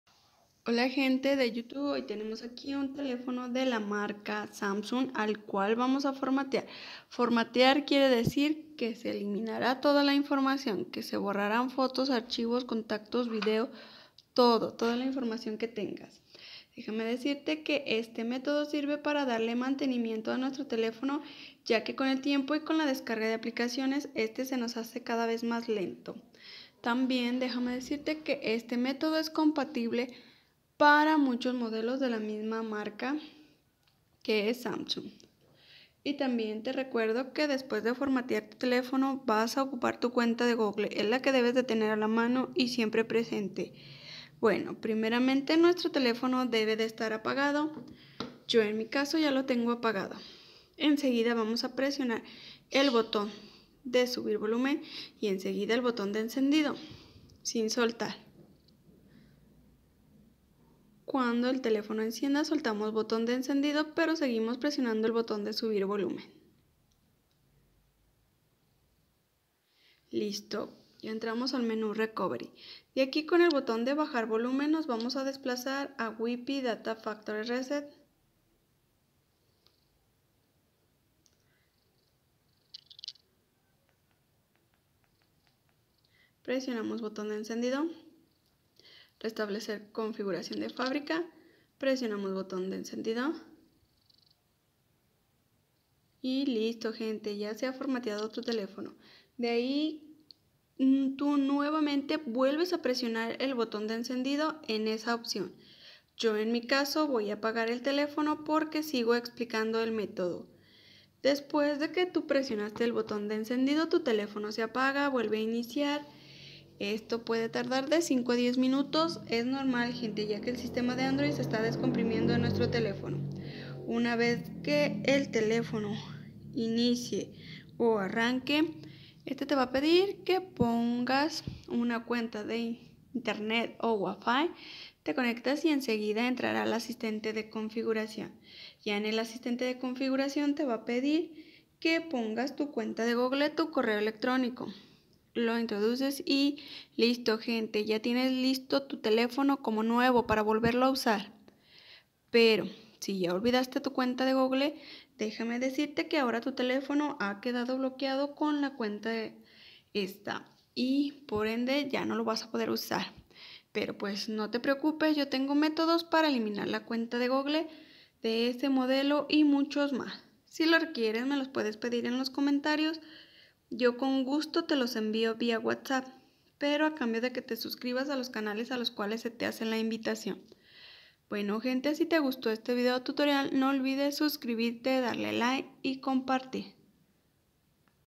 teléfono Samsung Galaxy falla, no abre apps, se reinicia de repente como formatear
usuario: @jejou